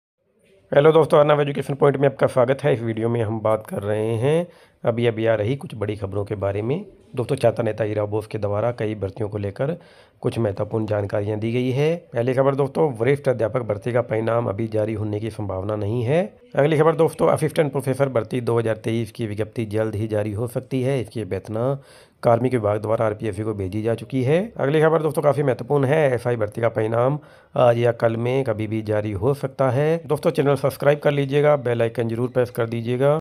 अच्छी खबर - नई भर्ती की विज्ञप्ति तथा रिजल्ट अपडेट | Rajasthan new vacancy and results update today
is video me Rajasthan ki new vacancy aur results se related latest update Di hai